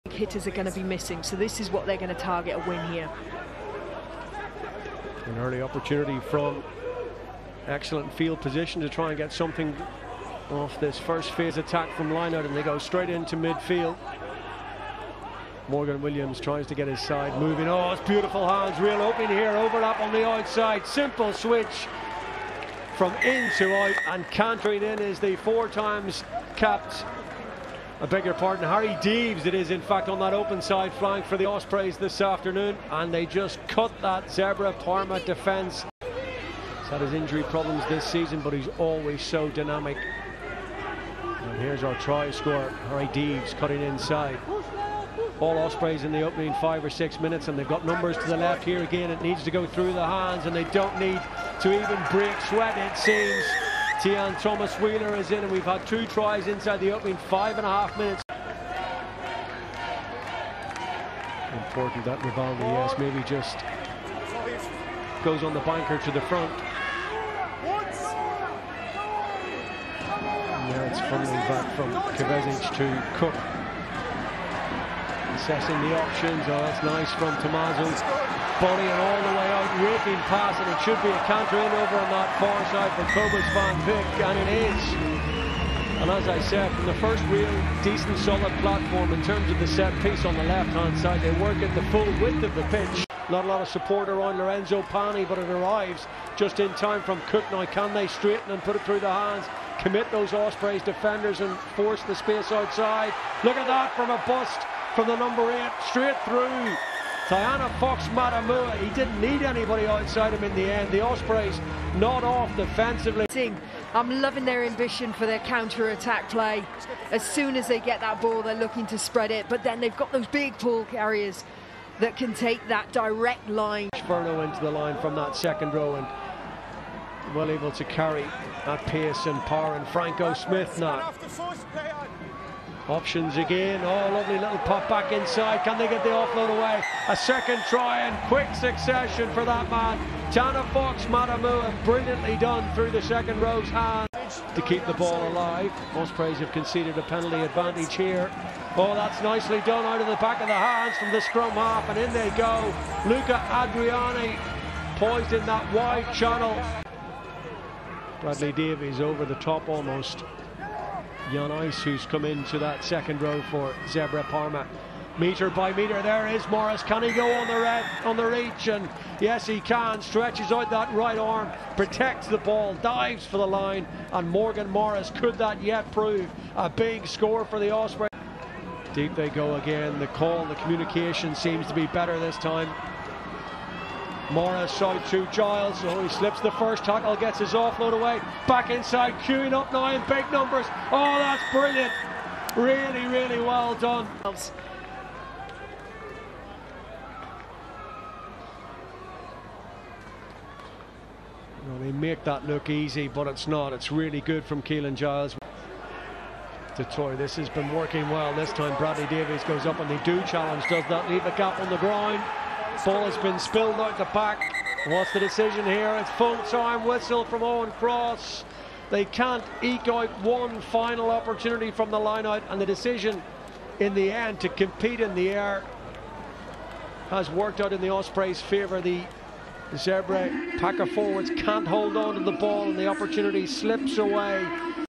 Zebre Parma v Ospreys | Instant Highlights | Round 13 | URC 2022/23
00:00:00 - Long Try by Harri Deaves
00:00:36 - Try by Tiaan Thomas-Wheeler
00:00:47 - Try Assist by Michael Collins
00:00:58 - Long Try by Jacobus van Wyk
00:01:33 - Try Assist by Josh Furno
00:02:04 - Try by Taina Fox-Matamua
00:02:24 - Try by Luca Andreani
00:02:37 - Try by Morgan Morris
00:02:58 - Try by Reuben Morgan-Williams
00:03:25 - Match End by Zebre

Watch extended highlights of the action as Zebre Parma took on the Ospreys in ROUND 13 of the BKT United Rugby Championship.